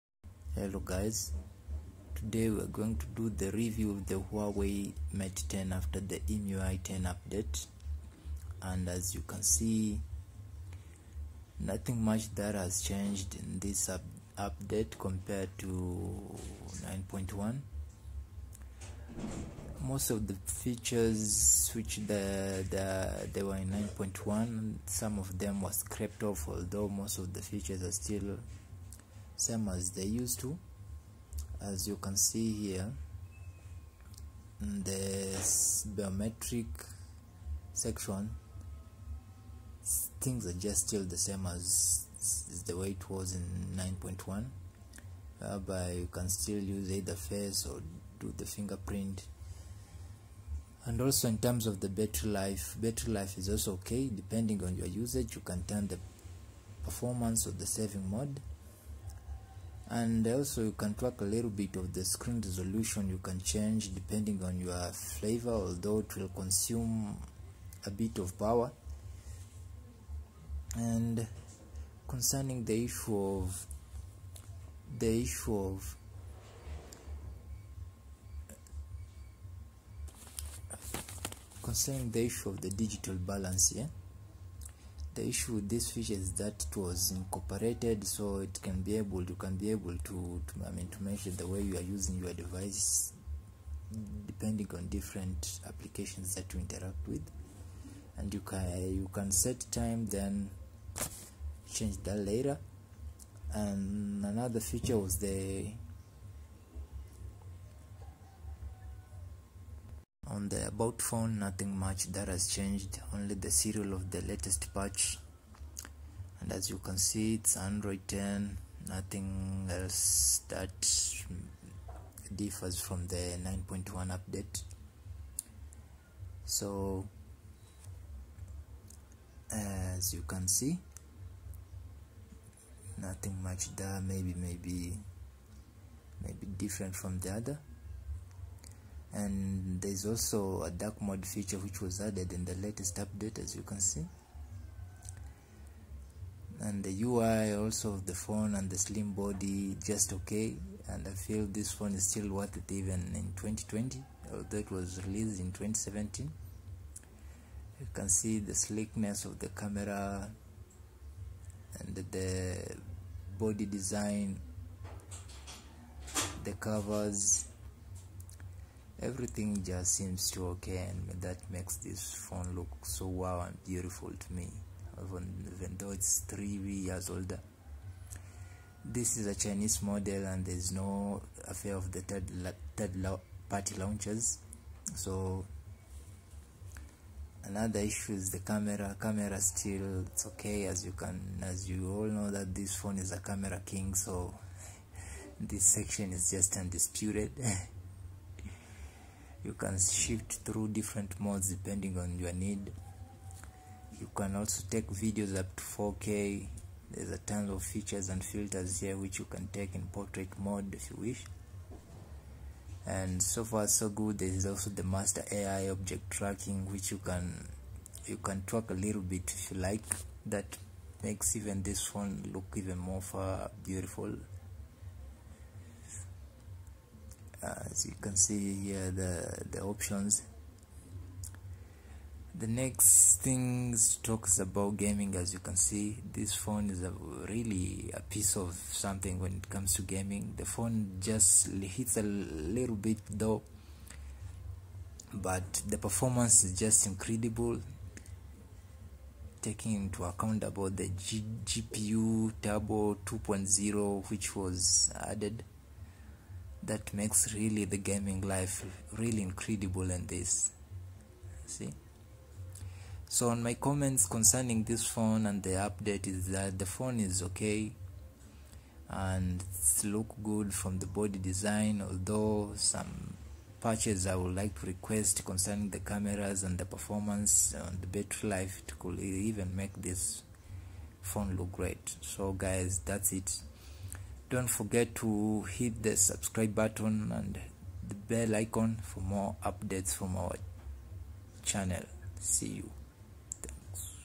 Emui 10 mate 10 After Update Review
Wondering What changed,  What was included From User Interface to Performance after Android 10 then you're at right place. Full review all included